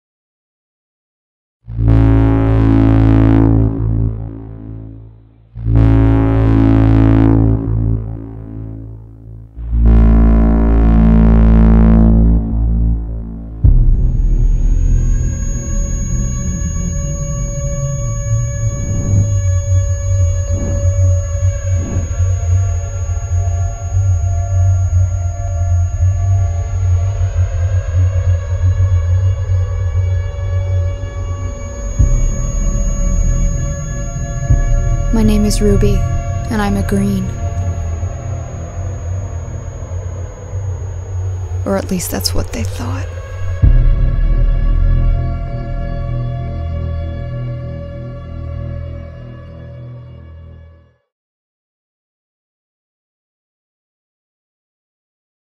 The Darkest Minds by Alexandra Bracken Trailer Teaser
Teaser trailer for 'The Darkest Minds'.

Please note: This is not an official teaser trailer for 'The Darkest Minds', they are adaptations from the novels and turned into short films made by fans of the books and are in no way affiliated with the author or publisher (although we do hope you enjoy it)

Piéra Forde as Ruby

Psi Kids:
Aimee Duroux
Zoe Platt
Molly Williams
Taryn Saarman
Cait Lowe
Lauren Daly

Adapted and Directed by Piéra Forde.
Filmed and Edited by Dale Norton.
Director of Photography: Jacob Irvine.

'The Darkest Minds' series by Alexandra Bracken.